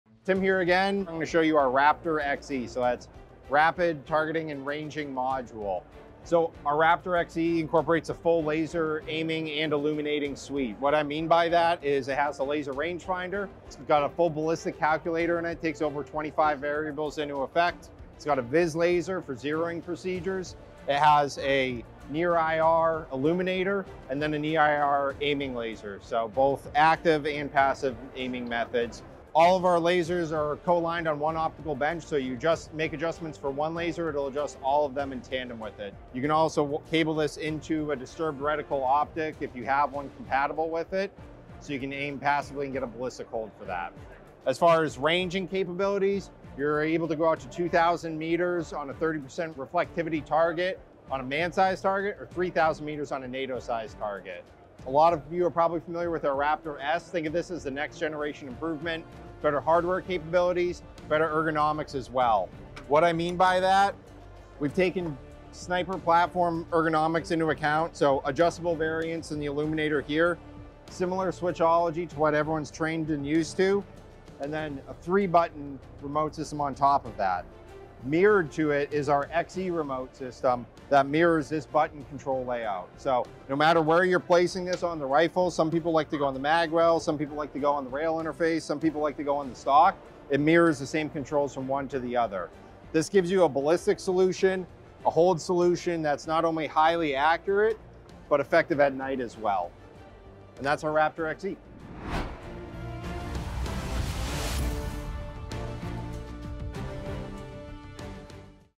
RAPTAR Xe - Rapid Targeting and Ranging Module ENHANCED
Our Customer Service Rep, Tim, gives an up-close look at the Wilcox RAPTAR Xe. Building on the legacy of the RAPTAR S, the RAPTAR Xe is a smaller, lighter, stronger laser aiming device with leading edge features that give snipers and tactical shooters increased accuracy a precise engagement at extremely long ranges.